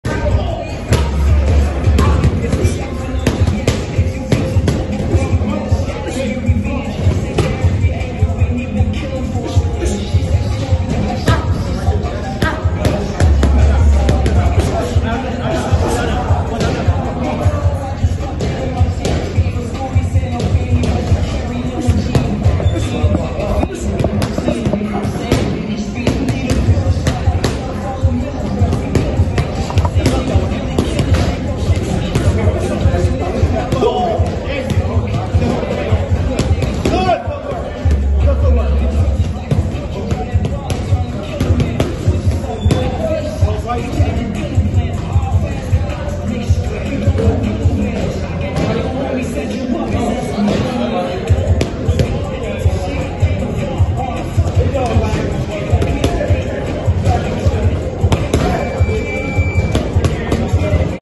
Curmel Moton at the Mayweather gym with real deal boxing talk putting that work in…..
Curmel Moton the next star in boxing putting in work at the Mayweather gym with Real Deal Boxing Talk.